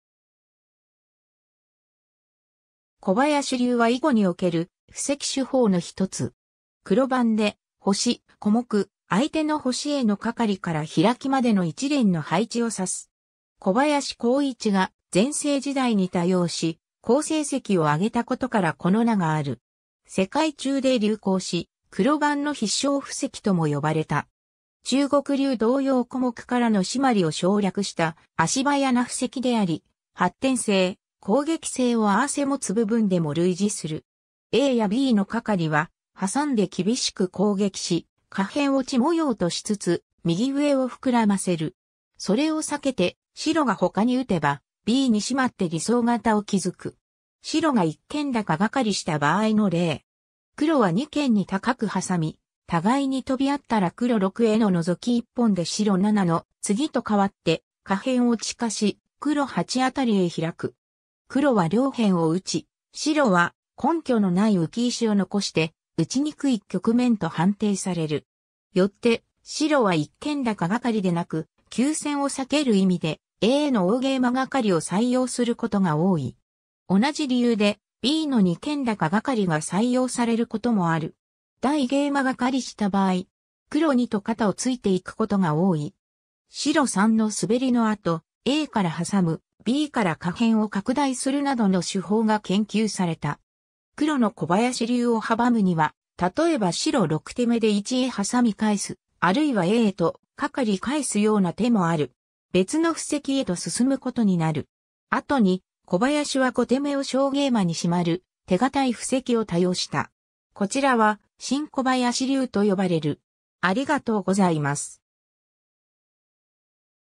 小林流 (囲碁)
小林流（こばやしりゅう）は囲碁における布石手法の一つ。
黒番で星・小目、相手の星へのカカリからヒラキまでの一連の配置を指す。
小林光一が全盛時代に多用し、好成績を挙げたことからこの名がある。
世界中で流行し、黒番の必勝布石とも呼ばれた。
 中国流同様小目からのシマリを省略した足早な布石であり、発展性・攻撃性を併せ持つ部分でも類似する。
 aやbのカカリは挟んで厳しく攻撃し、下辺を地模様としつつ右上をふくらませる。
それを避けて白が他に打てば、bにシマって理想形を築く。
 白が一間高ガカリした場合の例。
黒は二間に高くハサみ（村正の妖刀）、互いにトビ合ったら黒6へのノゾキ一本で白7のツギと換わって下辺を地化し、黒8あたりへヒラく。
黒は両辺を打ち、白は根拠のない浮き石を残して打ちにくい局面と判定される。
よって白は一間高ガカリでなく、急戦を避ける意味でaの大ゲイマガカリを採用することが多い。
同じ理由でbの二間高ガカリが採用されることもある。
 大ゲイマガカリした場合。
黒2と肩を突いていくことが多い。
白3のスベリの後、aからハサむ、bから下辺を拡大するなどの手法が研究された。
 黒の小林流を阻むには、例えば白6手目で1へハサミ返す、あるいはaへとカカリ返すような手もある。
別の布石へと進むことになる。
 後に小林は5手目を小ゲイマにシマる手堅い布石を多用した。
こちらは「新小林流」と呼ばれる。